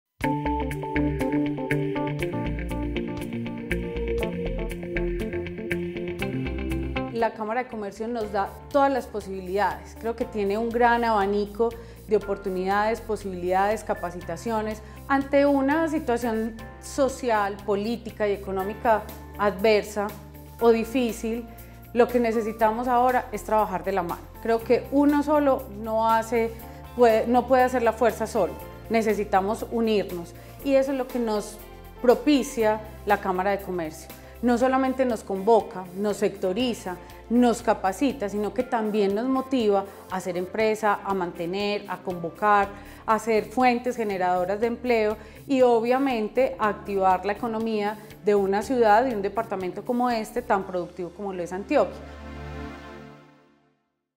JuntosHacemosEmpresa - Testimonio Empresarial Verónica Jaramillo - Los Halcones S.A.S
JuntosHacemosEmpresa. Acompañar a nuestros empresarios en su proceso de crecimiento es uno de los principales motivos por los que trabajamos. En la Cámara de Comercio destacamos el compromiso y esfuerzo de quienes apoyan al país a través de la creación de empresa 👏